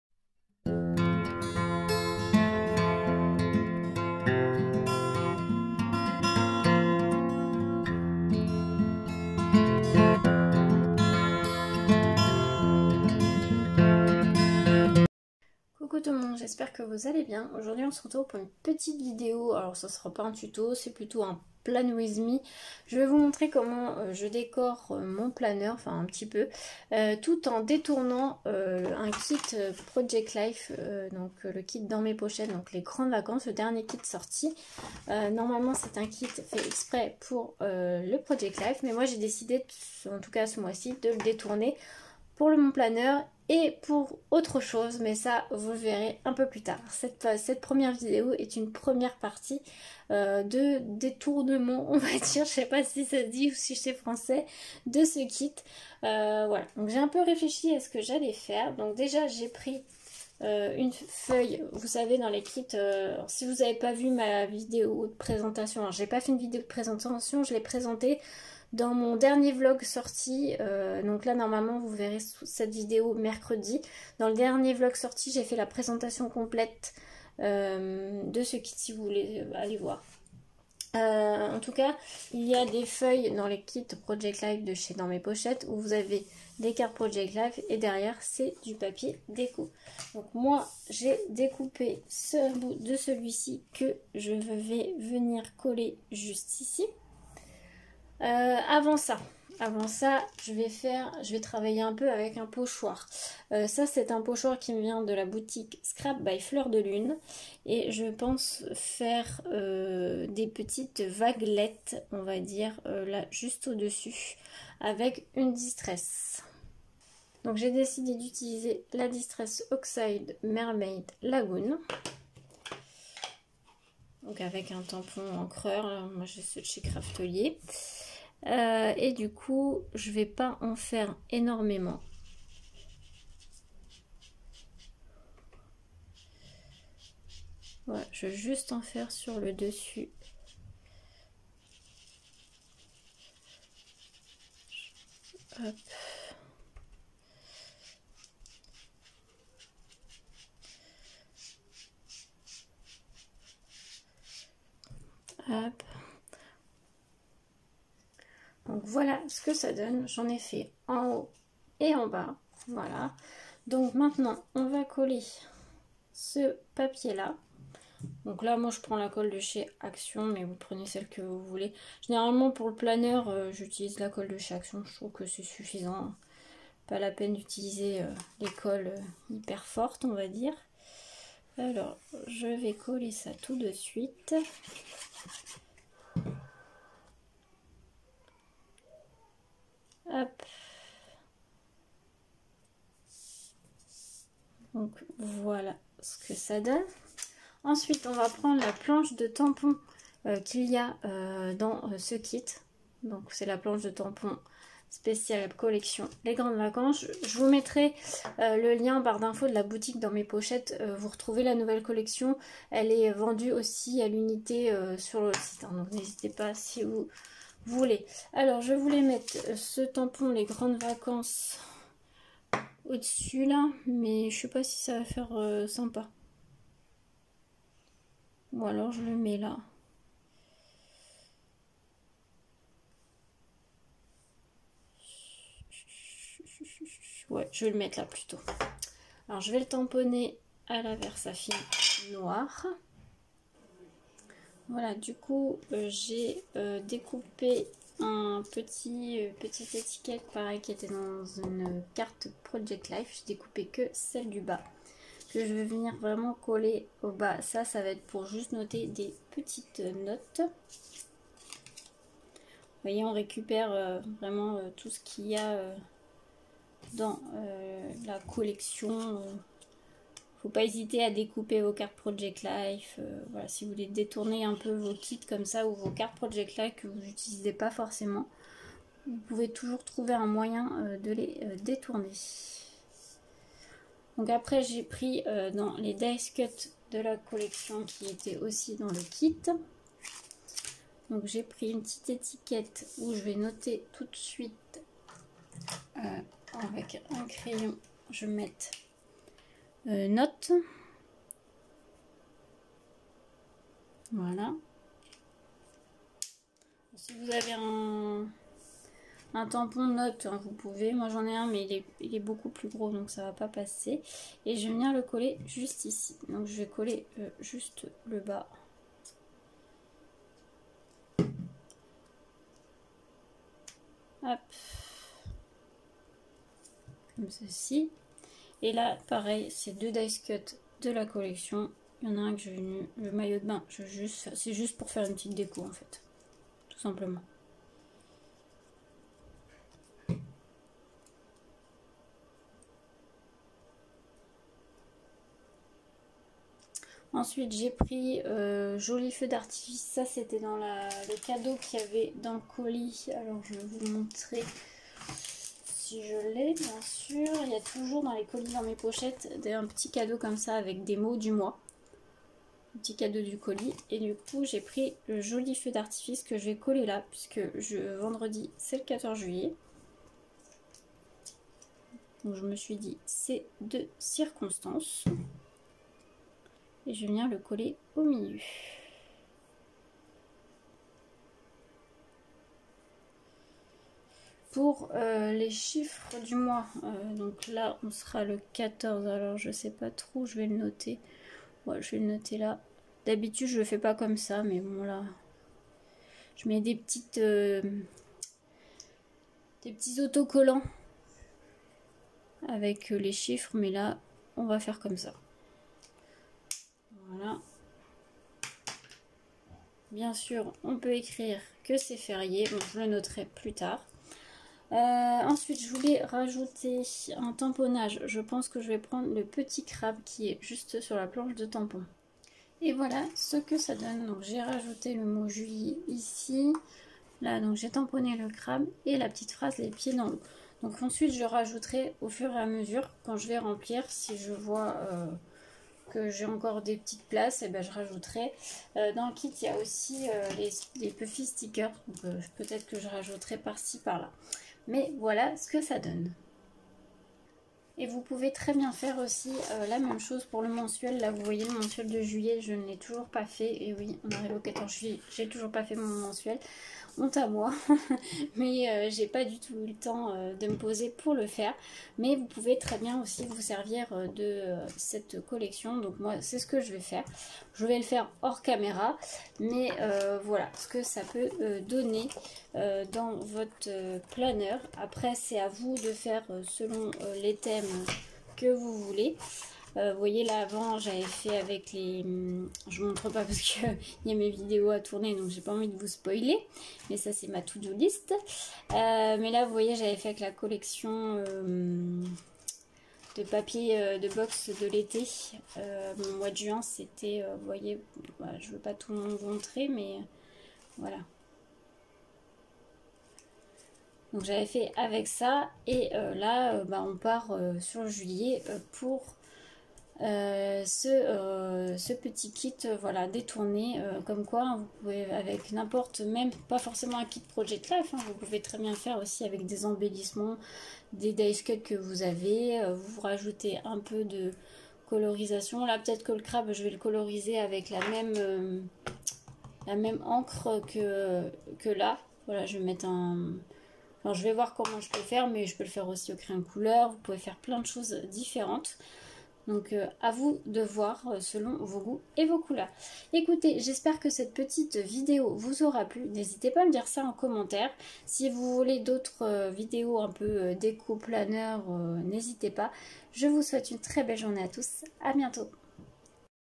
planwithme On détourne un kit...
Coucou
Dans cette vidéo on va détourner un kit Project life de la boutique @dansmespochettesdescrap pour réaliser ma semaine dans mon planner
Bon visionnage et dites moi si ce style de vidéo vous plait...

ma page Facebook: Les loisirs d'elodie